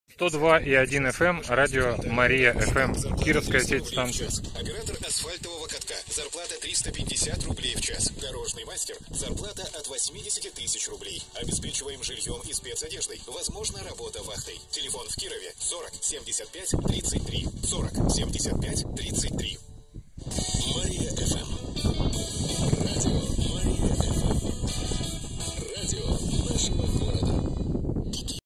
102.8FM МарияFM Яранск Кировская область 300вт, 24 км 30-04-2023
102.8FM МарияFM Яранск Кировская область 300Вт, 24 км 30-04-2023
Мощная станция, 30км по прямой, у меня в Йошкар-Оле не принимается - местный "киловаттник" сбоку давит...